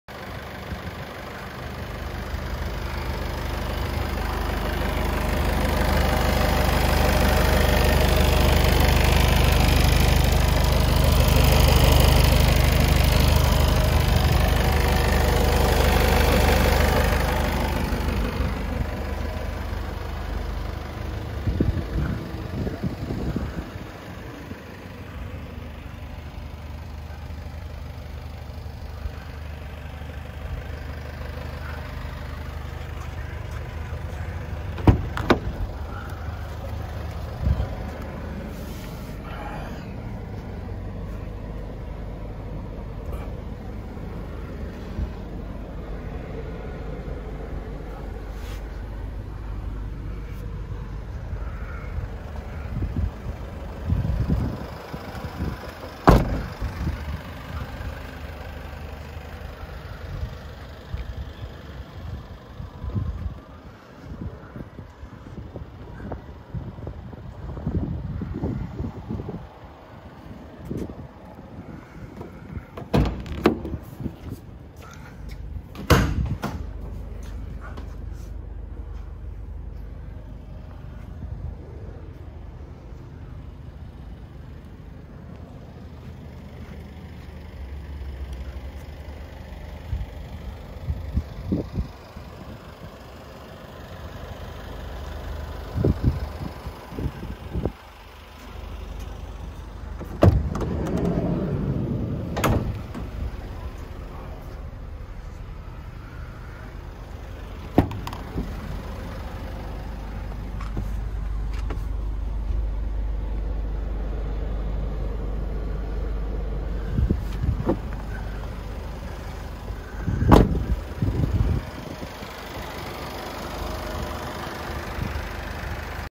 Citroen Relay 35 L3 H2 Enterprise 2.0 HDI (130), 2019 (19), 1 Owner, 2 x Keys, FSH, Bluetooth / Music Streaming, DAB, Cruise Control & Speed Limiter, Bulkhead, Side Loading Door, Electric Windows & Mirrors, 3 x Seats, Euro6, 63,000 miles, £17,490 + VAT - LW19 ETA